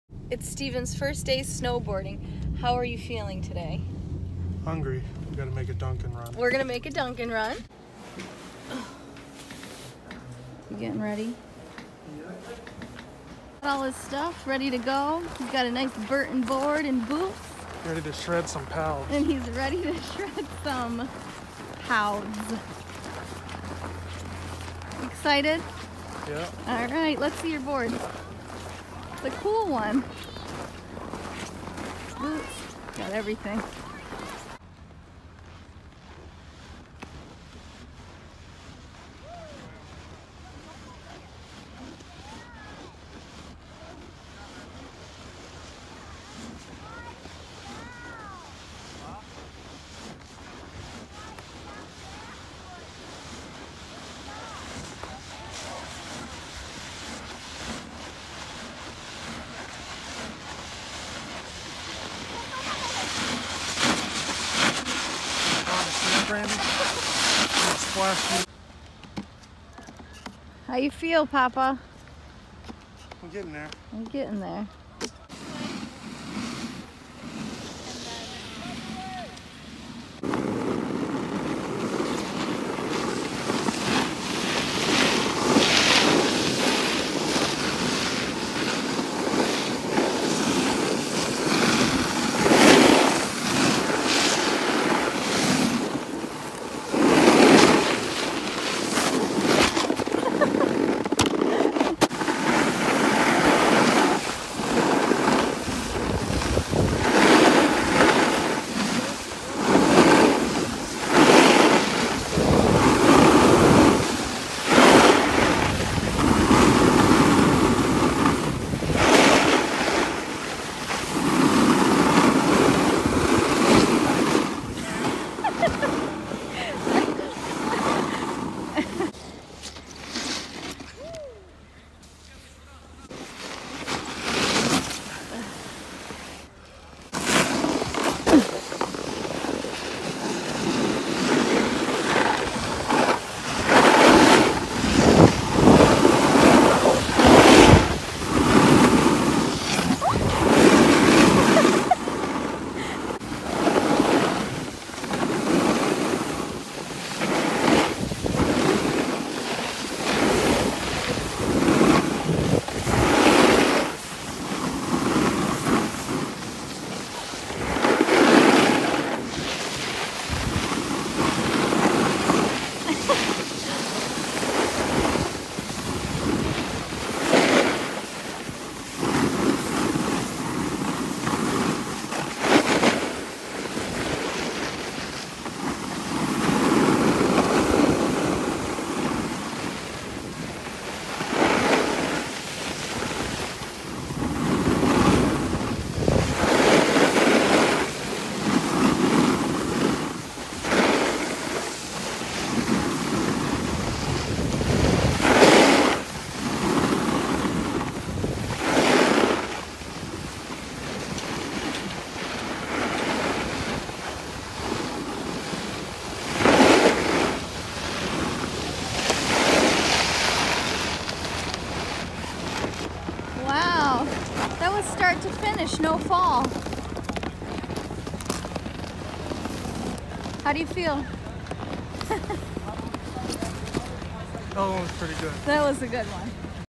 First Time Snowboarding!
Big Papa takes on the Bunny Hill